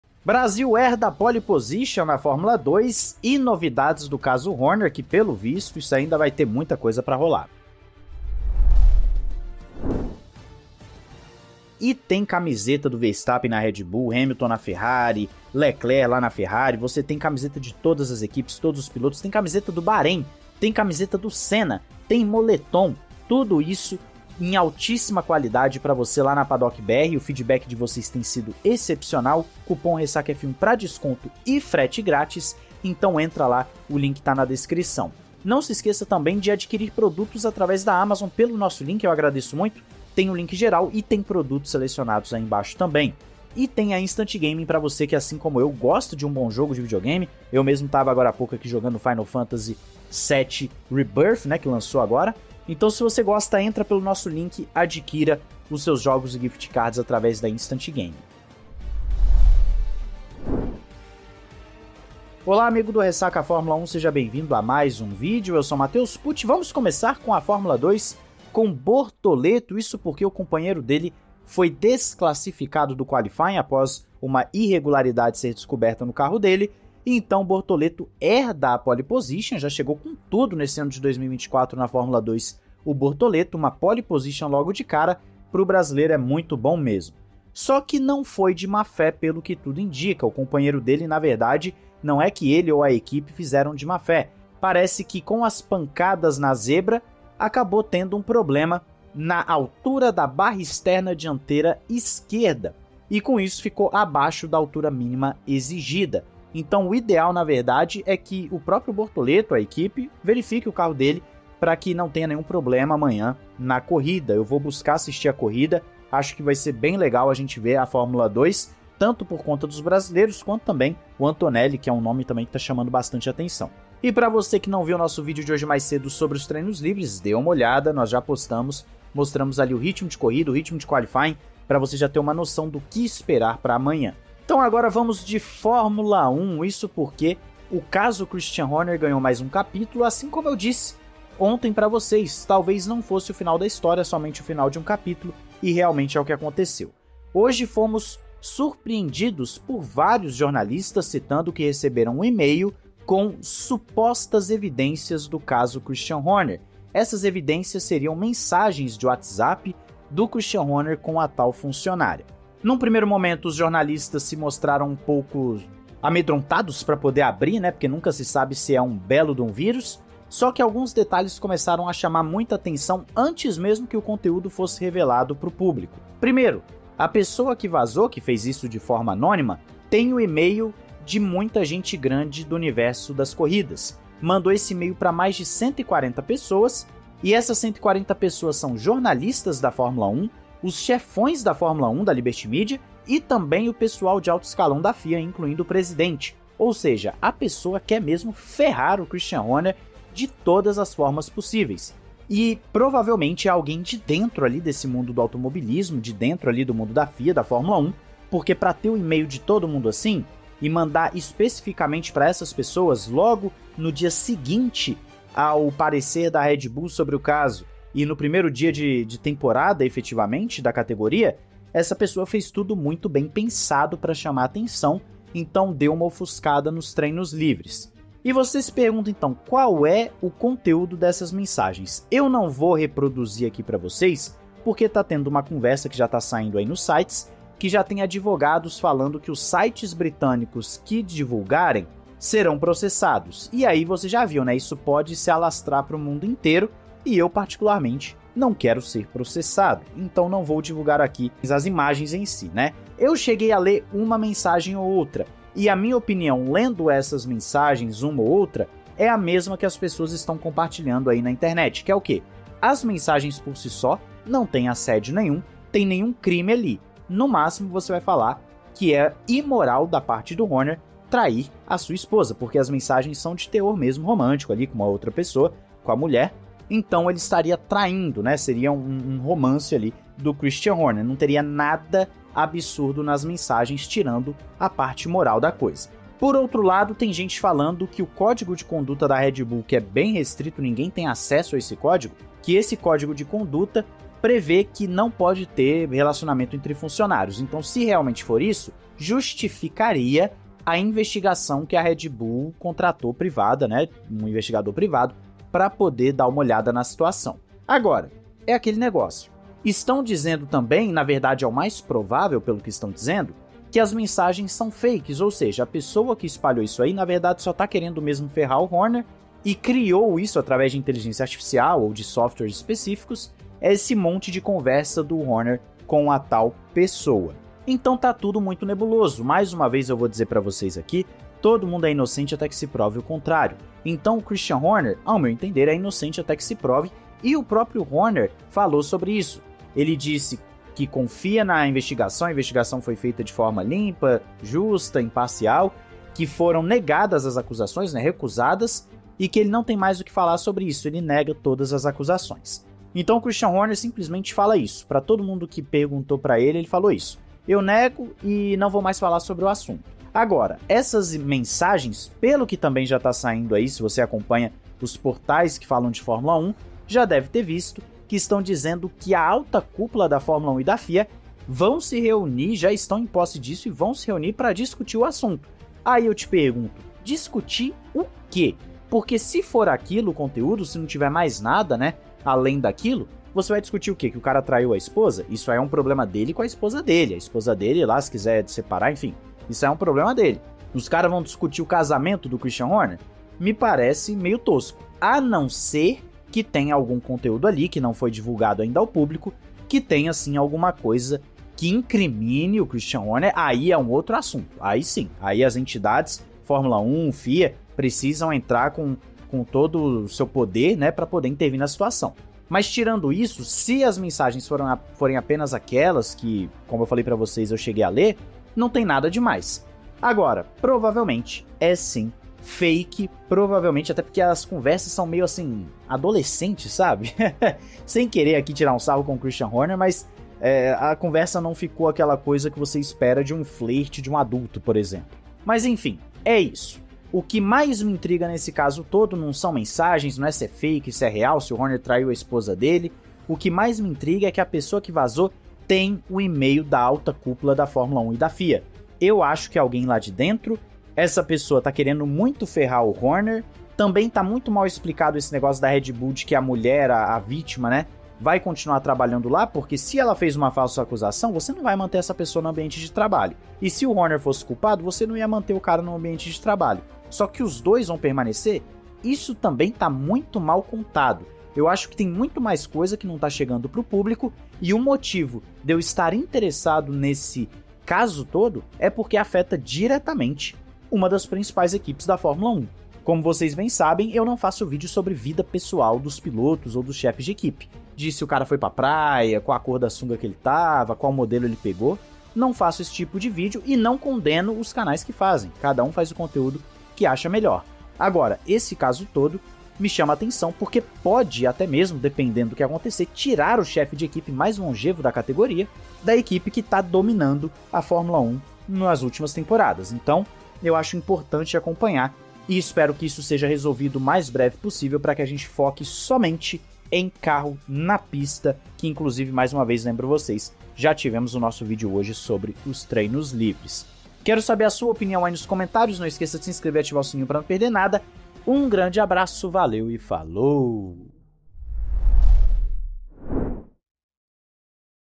CASO HORNER BOMBANDO E GABRIEL BORTOLETO POLE POSITION!
Sergio Milani
@scuderiamilani (Twitter)

📰 *Fontes:*

João 3:16

00:00 - Introdução
01:03 - Bortoleto POLE
02:18 - Novo capítulo do caso Horner
03:49 - Nada demais a principio
05:40 - Horner se defende
06:15 - FIA e F1 Vão interferir no casamento dele?
07:18 - Alguém tem o contato de todo mundo da FIA e F1...